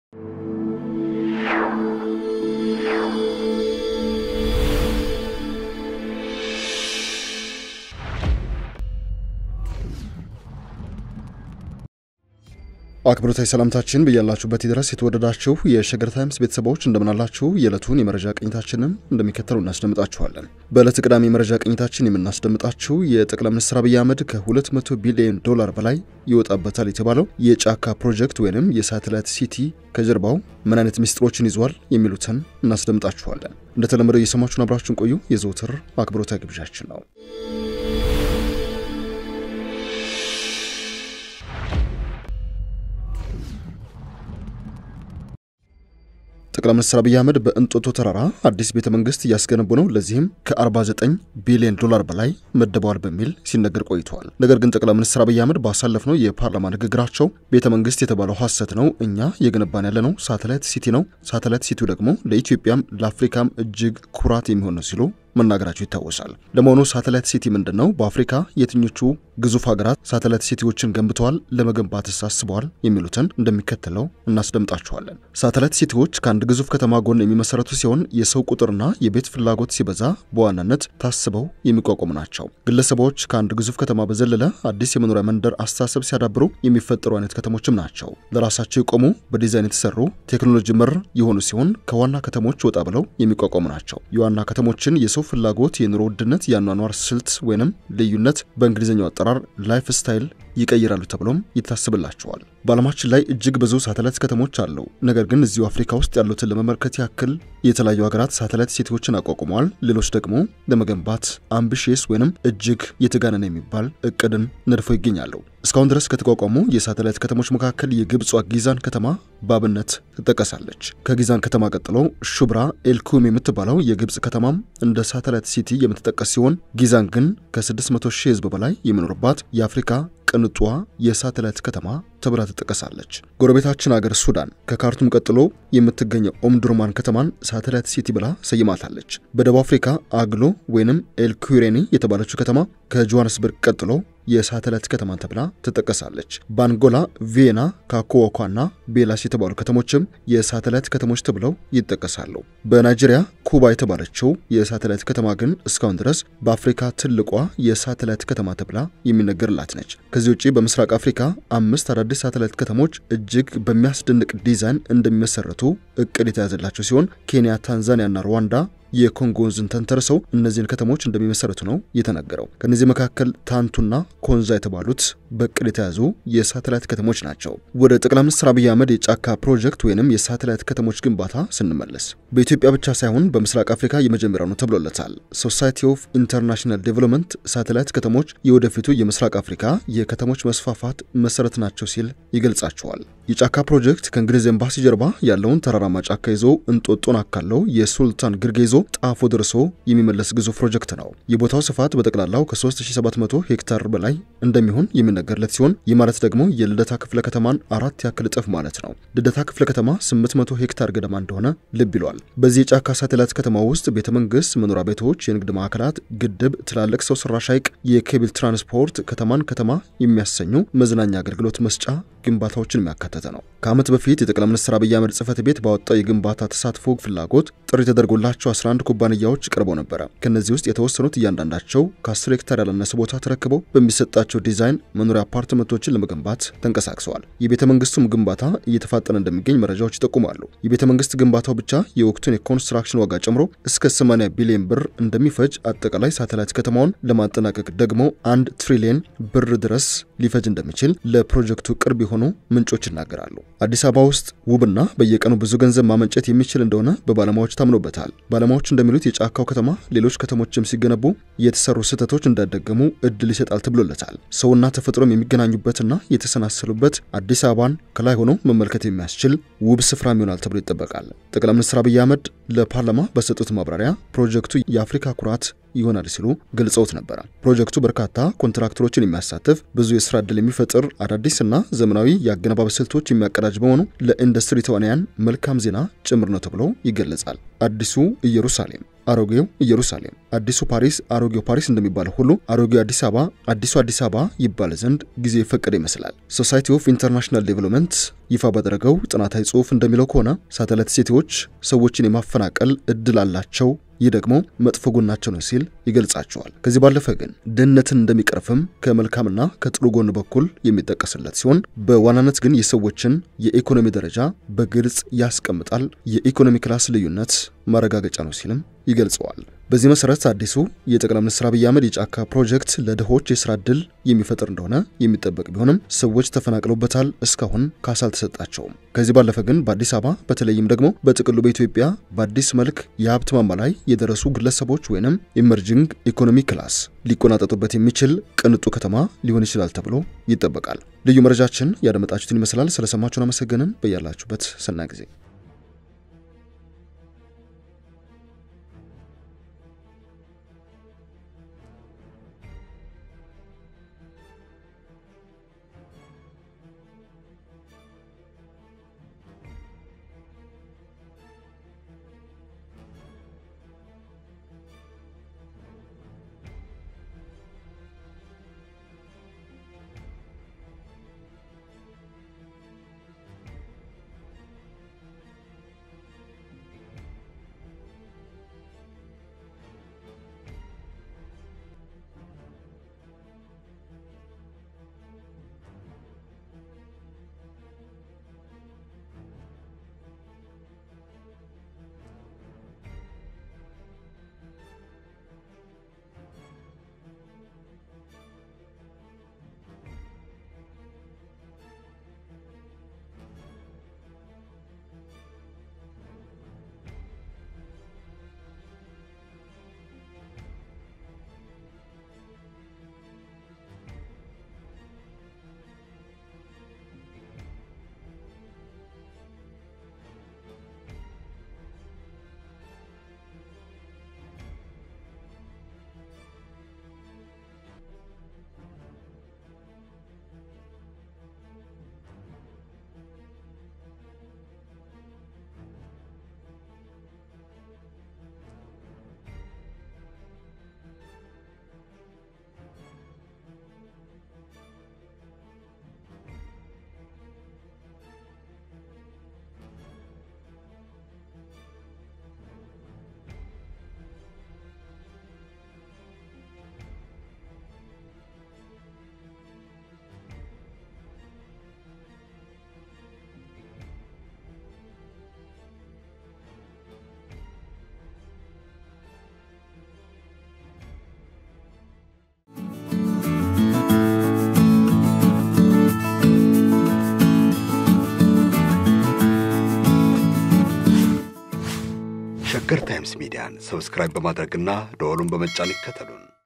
Ethiopia:ልዩ መረጃ|የጫካው ከተማ ፕሮጀክት ምስጢር ሲገለጥ..!|1 ትሪለየን ብር ወጪ..!|3700 ሄክታር..!|Sheger Times Media
Ethiopia:ሰበር|የጫካው ከተማ ፕሮጀክት ምስጢር ሲገለጥ..!|1 ትሪለየን ብር ወጪ..!|3700 ሄክታር..!|Sheger Times Media